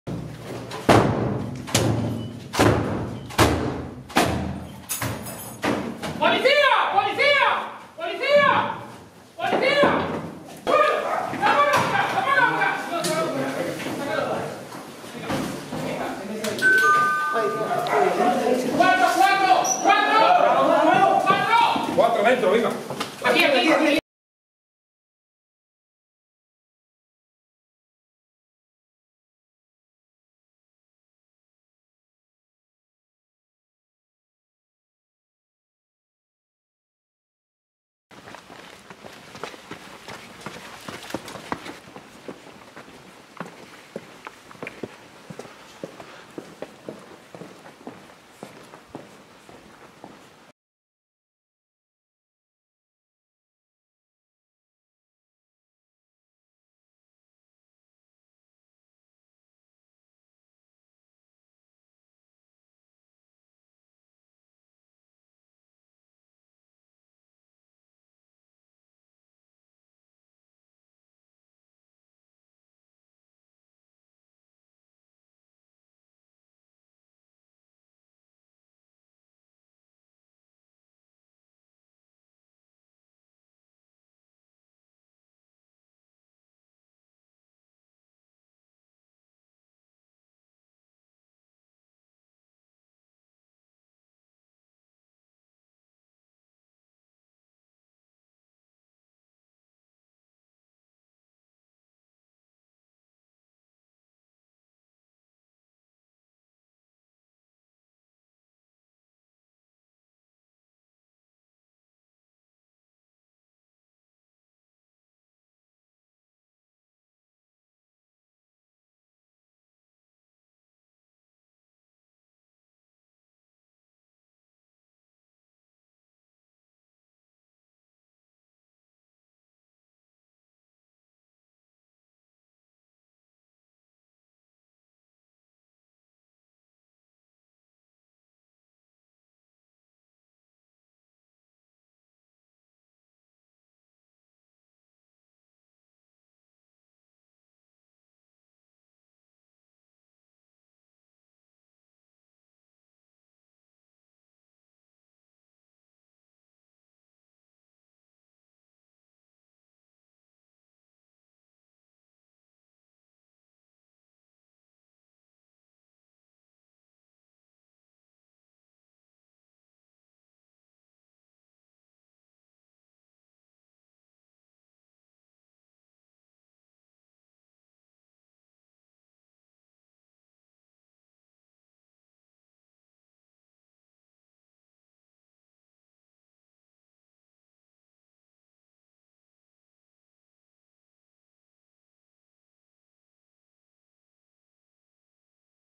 Desarticulado un entramado dedicado a la comercialización ilegal de canales de televisión de pago
Daban servicio a alrededor de 20.000 clientes

La Policía Nacional desarticula un entramado dedicado a la  comercialización ilegal de canales de televisión de pago y vídeos bajo demanda

En esta operación han sido detenidas 12 personas, cuatro de ellas encargadas de la obtención de la señal televisiva y de su posterior difusión ilegal, además de publicitar este servicio, captar clientes y realizar los cobros

Se han cerrado siete páginas web y dos perfiles en redes sociales donde se anunciaban los servicios ilícitos, y se han intervenido 86 decodificadores de señal de televisión, 15 discos duros, 10 ordenadores, 17 teléfonos móviles, un vehículo de alta gama, 2 servidores NAS y 22.919 euros en efectivo